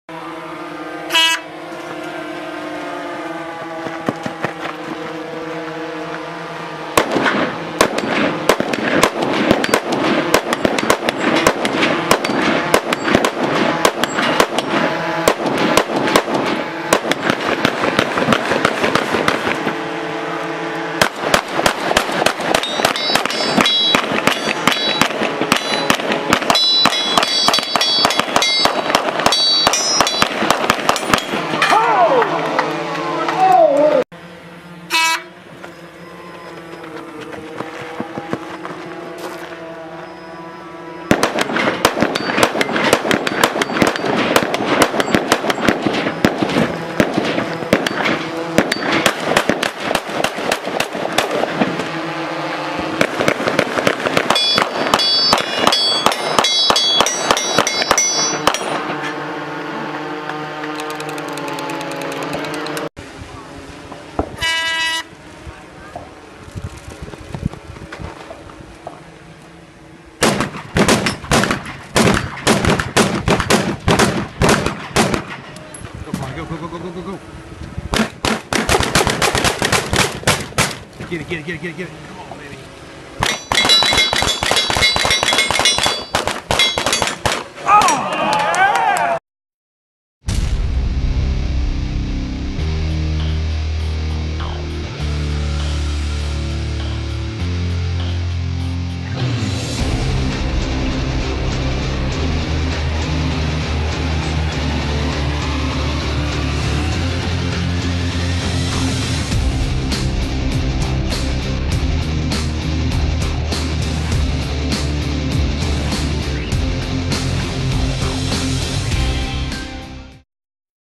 2014 3 Gun Nation Pro Series Rumble On The Range $50,000 Shoot Off Kalani Laker
Team Stag Arms Jesse Tischauser Shooting on the 3 Gun Nation Pro Series.